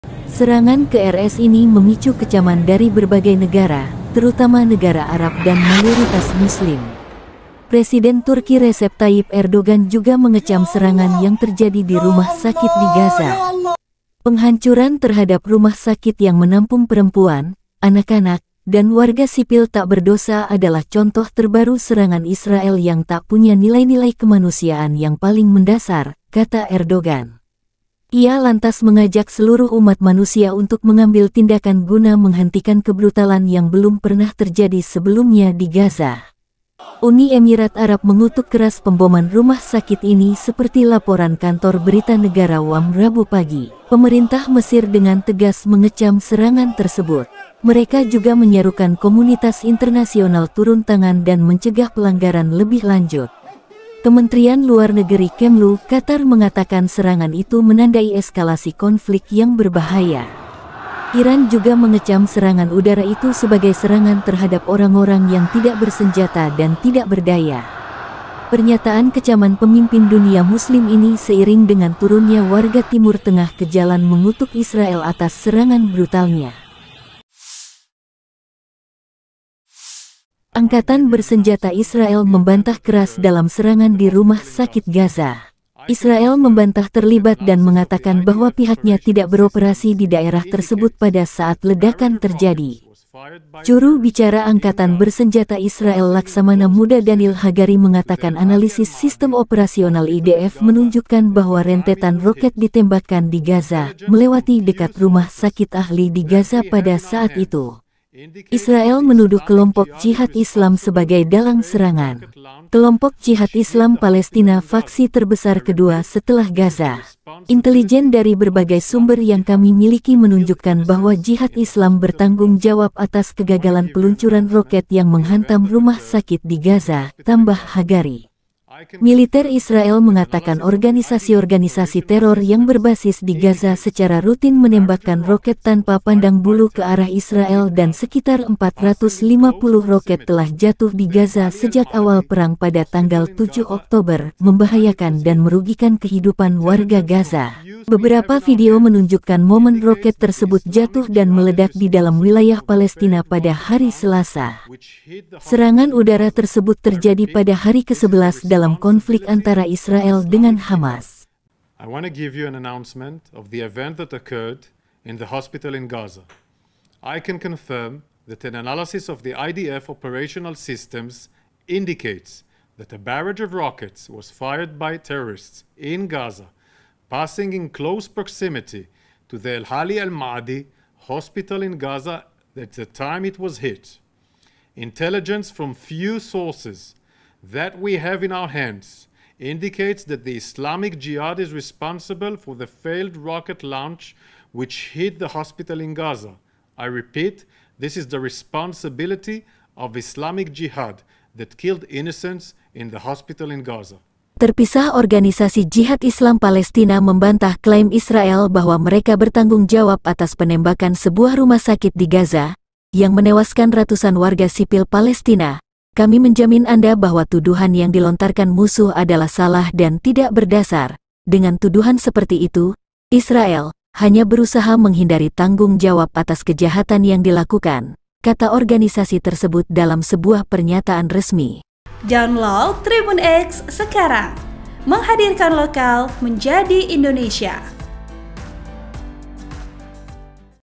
🔴 Erdogan Ajak Umat Manusia Bersatu Kutuk Israel atas Pemboman RS Gaza!
TRIBUN-TIMUR.COM - Presiden Turki Recep Tayyip Erdogan juga mengecam serangan yang terjadi di rumah sakit di Gaza.

Penghancuran terhadap rumah sakit yang menampung perempuan, anak-anak, dan warga sipil tak berdosa adalah contoh terbaru serangan Israel yang tak punya nilai-nilai kemanusiaan yang paling mendasar, kata Erdogan.

Ia lantas mengajak seluruh umat manusia untuk mengambil tindakan guna menghentikan kebrutalan yang belum pernah terjadi sebelumnya di Gaza.(*)